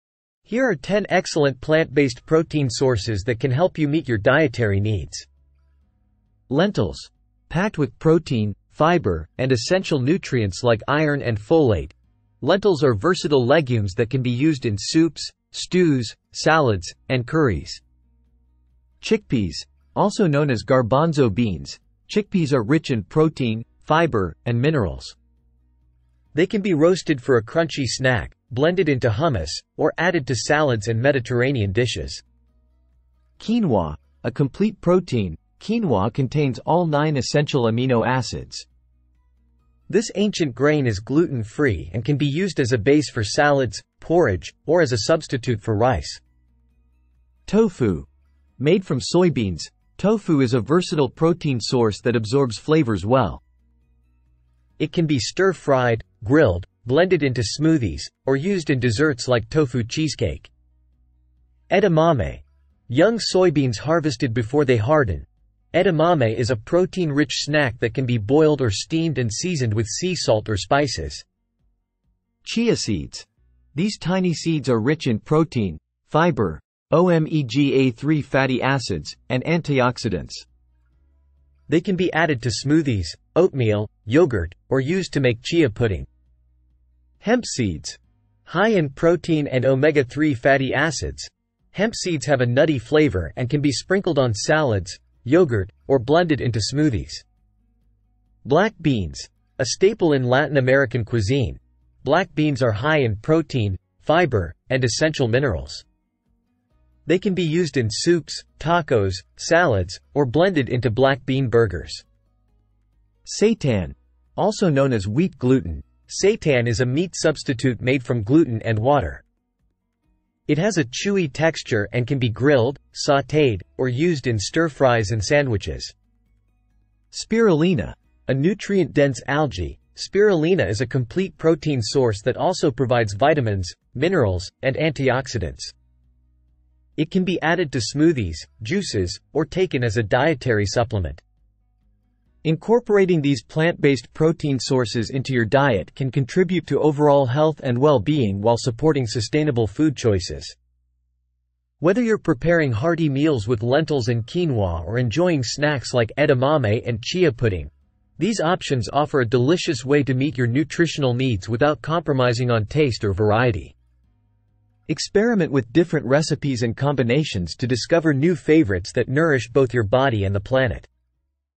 Top 10 Plant Based Protein Sources
Discover the best plant-based protein sources with our top 10 list, featuring nutritious and delicious options that will help you maintain a healthy and balanced diet without meat.